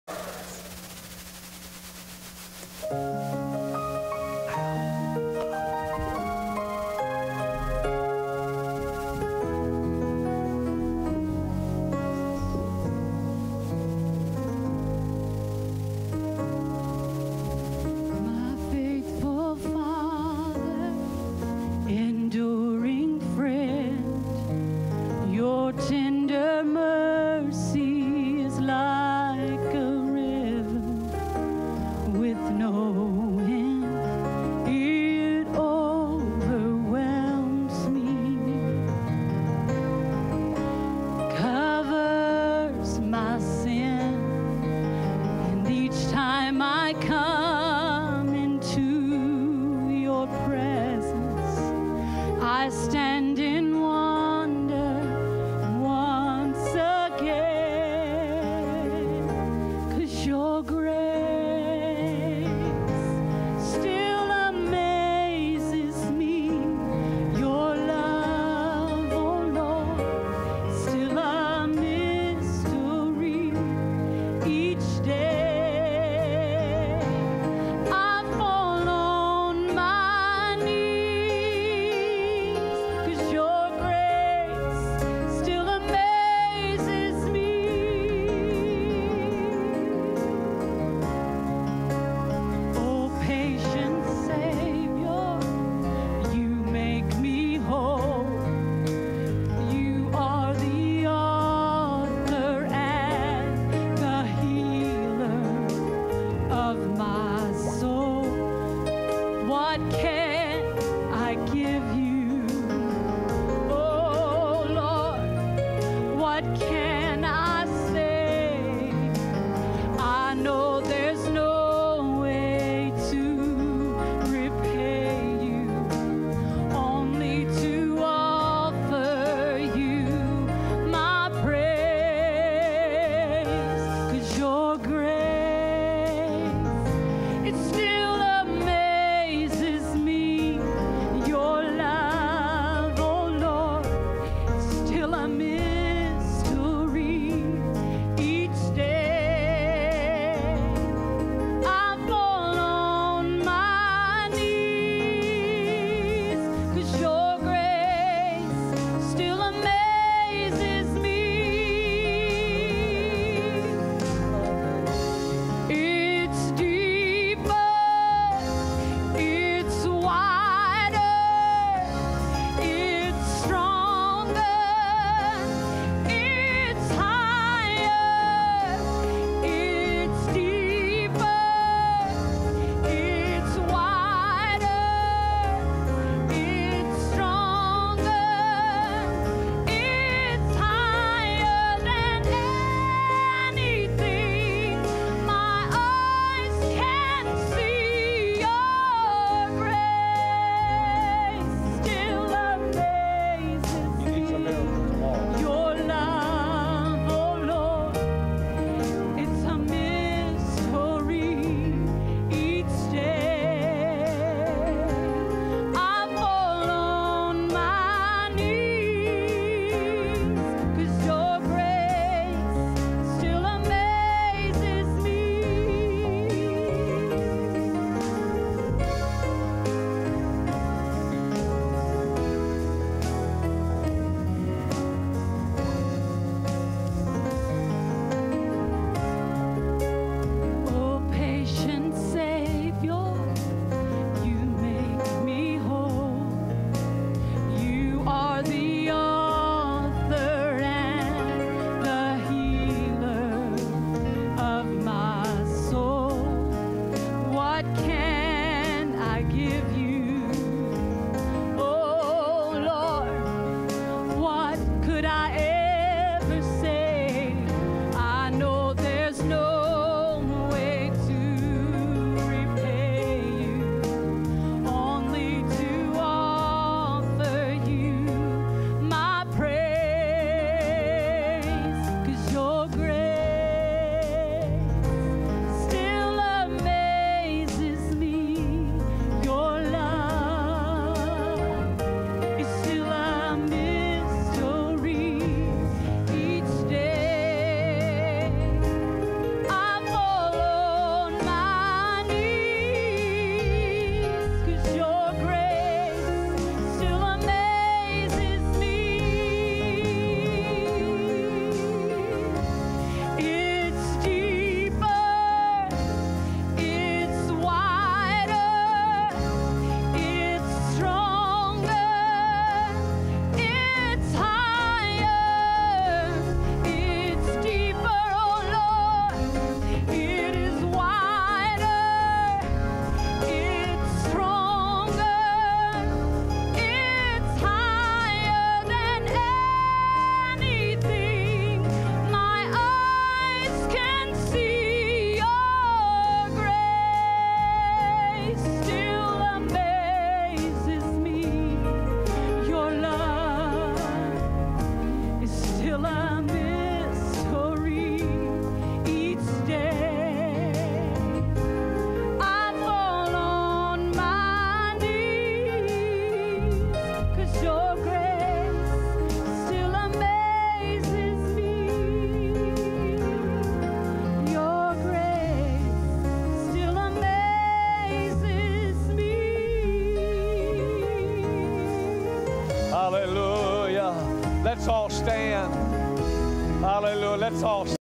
FPC of Wichita,KS - Sis. Carroll
Sis. Carroll singing in our Sunday Evening service on 11-10-13!!